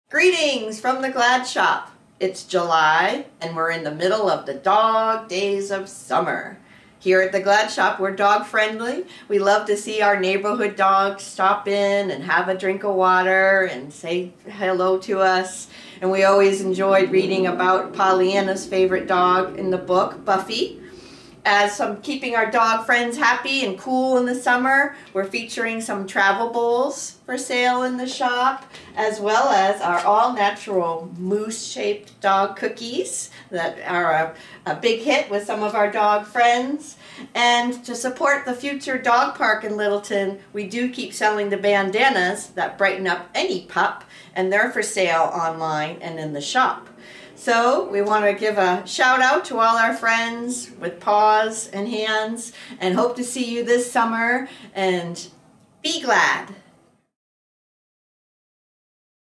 The Dog Days of Summer in Glad Town
It's July and and Summer is in full swing in Littleton!  We're celebrating the dog days of Summer at the Glad Shop with special products for our doggie friends!   We're always glad to see doggies in the window here!